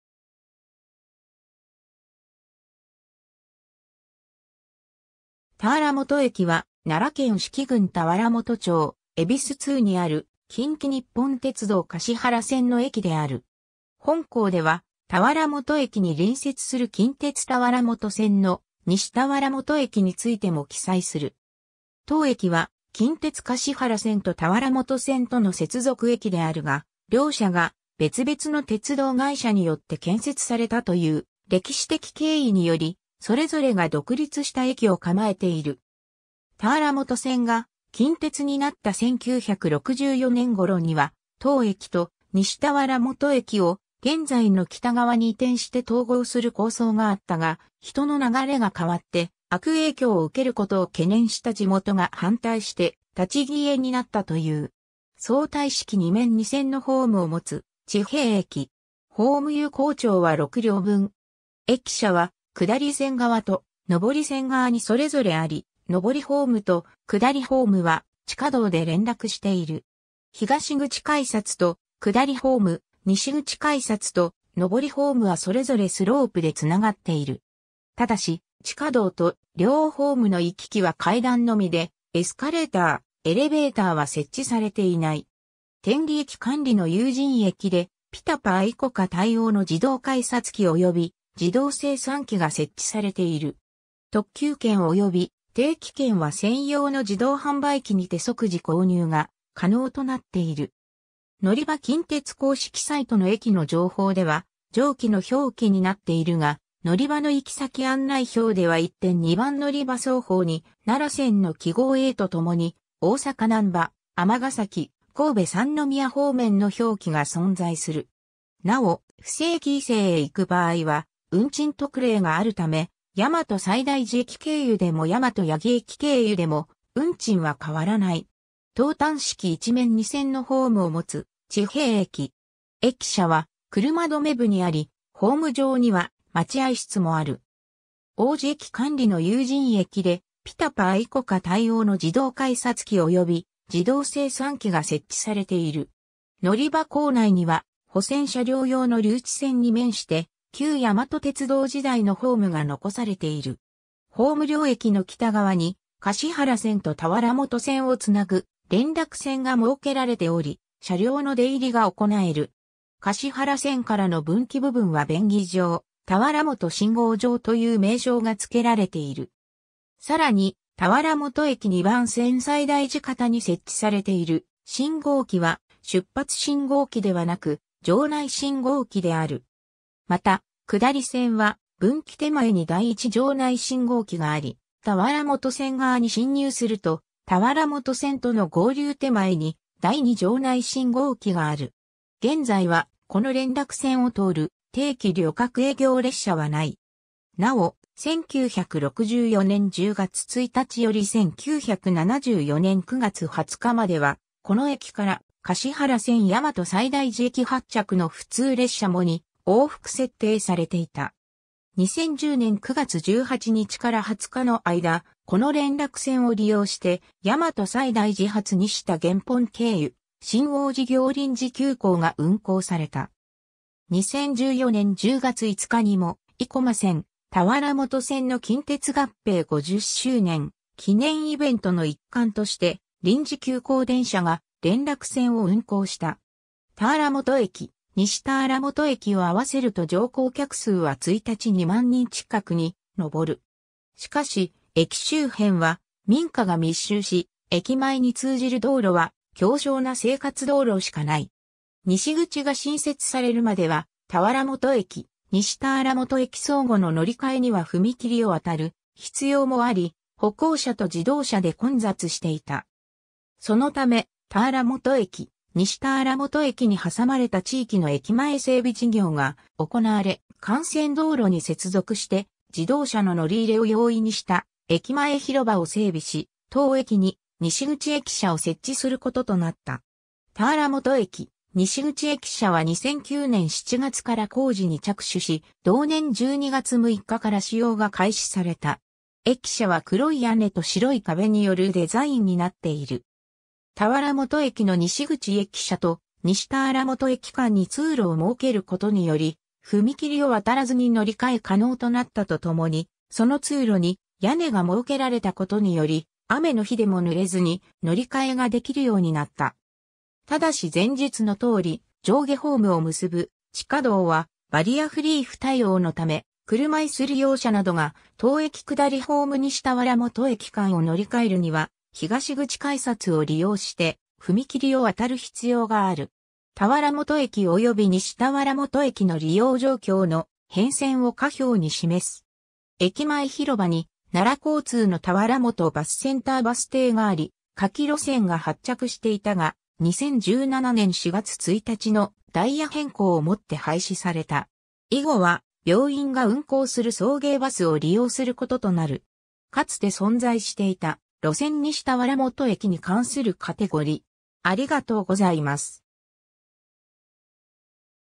田原本駅
田原本駅（たわらもとえき）は、奈良県磯城郡田原本町戎通にある、近畿日本鉄道（近鉄）橿原線の駅である。
 本項では、田原本駅に隣接する近鉄田原本線の西田原本駅（にしたわらもとえき）についても記載する。
 当駅は近鉄橿原線と田原本線との接続駅であるが、両者が別々の鉄道会社（大阪電気軌道と大和鉄道）によって建設されたという歴史的経緯により、それぞれが独立した駅を構えている。
田原本線が近鉄になった1964年頃には、当駅と西田原本駅を現在の北側に移転して統合する構想があったが、人の流れが変わって悪影響を受けることを懸念した地元（駅周辺の商店など）が反対して立ち消えになったという。
 相対式2面2線のホームを持つ地平駅。
ホーム有効長は6両分。
駅舎は下り線側（東口）と上り線側（西口）にそれぞれあり、上りホームと下りホームは地下道（改良工事前は構内踏切）で連絡している。
東口改札と下りホーム、西口改札と上りホームはそれぞれスロープでつながっている。
ただし、地下道と両ホームの行き来は階段のみで、エスカレーター、エレベーターは設置されていない。
 天理駅管理の有人駅で、PiTaPa・ICOCA対応の自動改札機および自動精算機（回数券カードおよびICカードのチャージに対応）が設置されている。
特急券および定期券は専用の自動販売機にて即時購入が可能となっている。
 のりば 近鉄公式サイトの駅の情報では上記の表記になっているが、のりばの行先案内標では1・2番のりば双方に奈良線の記号Aとともに「大阪難波・尼崎・神戸三宮方面」の表記が存在する。
なお、布施駅以西へ行く場合は運賃特例があるため、大和西大寺駅経由でも大和八木駅経由でも運賃は変わらない。
 頭端式1面2線のホームを持つ地平駅。
駅舎は車止め部にあり、ホーム上には待合室もある。
 王寺駅管理の有人駅で、PiTaPa・ICOCA対応の自動改札機および自動精算機（回数券カードおよびICカードのチャージに対応）が設置されている。
 のりば 構内には、保線車両用の留置線に面して旧大和鉄道時代のホームが残されている。
 ホーム 両駅の北側に橿原線と田原本線をつなぐ連絡線が設けられており、車両の出入りが行える。
橿原線からの分岐部分は便宜上「田原本信号場」という名称が付けられている。
さらに、田原本駅2番線（上り線）西大寺方に設置されている信号機は（田原本駅からの）出発信号機ではなく、（田原本信号場への）場内信号機である。
また、下り線は分岐手前（大和西大寺方）に第一場内信号機（橿原線/田原本線）があり、田原本線側に進入すると、田原本線との合流手前に第二場内信号機（一号線/二号線）がある。
 現在は、この連絡線を通る定期旅客営業列車はない。
なお、1964年10月1日より1974年9月20日まではこの駅から橿原線大和西大寺駅発着の普通列車も2往復設定されていた。
2010年（平成22年）9月18日 - 20日の間、この連絡線を利用して大和西大寺発西田原本経由新王寺行臨時急行が運行された。
2014年10月5日(日)にも、生駒線・田原本線の近鉄合併50周年記念イベントの一環として、臨時急行電車が連絡線を運行した。
 田原本駅・西田原本駅を合わせると乗降客数は1日2万人近くにのぼる（田原本駅 13,000人、西田原本駅 5,000人で、乗換え客 4,000人も両駅の乗降人員に含む）。
しかし、駅周辺は民家が密集し、駅前に通じる道路は狭小な生活道路しかない。
西口が新設されるまでは、田原本駅・西田原本駅相互の乗り換えには踏切を渡る必要もあり（さらに田原本駅上りホームの利用には、地下道も通る必要がある）、歩行者と自動車で混雑していた。
 そのため、田原本駅・西田原本駅に挟まれた地域の駅前整備事業が行われ、幹線道路(奈良県道14号桜井田原本王寺線)に接続して自動車の乗り入れを容易にした駅前広場を整備し、当駅に西口駅舎を設置することとなった。
 田原本駅西口駅舎は2009年（平成21年）7月から工事に着手し、同年12月6日から使用が開始された。
駅舎は黒い屋根と白い壁によるデザインになっている。
 田原本駅の西口駅舎と西田原本駅間に通路を設けることにより、踏切を渡らずに乗り換え可能となったとともに、その通路に屋根が設けられたことにより、雨の日でも濡れずに乗り換えができるようになった。
ただし前述のとおり、上下ホームを結ぶ地下...